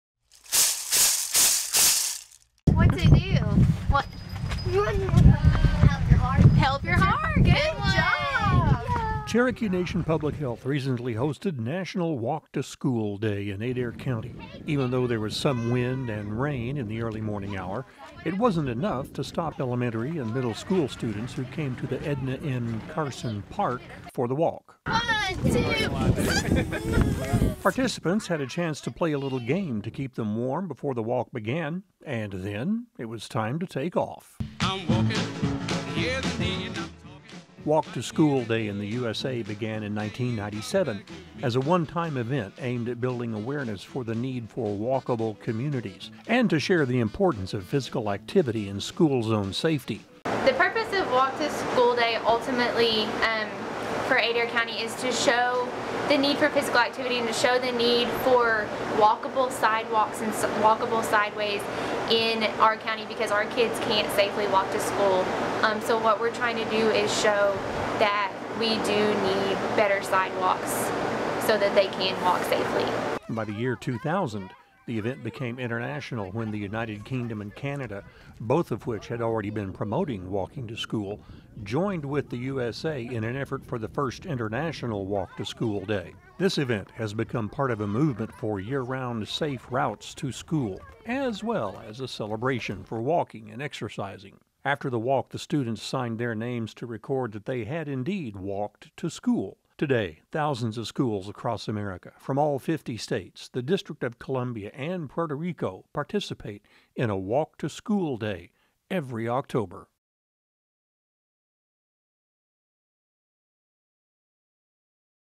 National Walk to School Day
Cherokee Nation Public Health hosted National Walk to School Day this year in Adair County.  Wind and rain did not stop Elementary and Middle school students who came to Edna M. Carson Park for the walk.

Walk to School Day in the USA began in 1997 as a one-day event aimed at building awareness for the need for walkable communities and to share the importance of physical activity and school zone safety.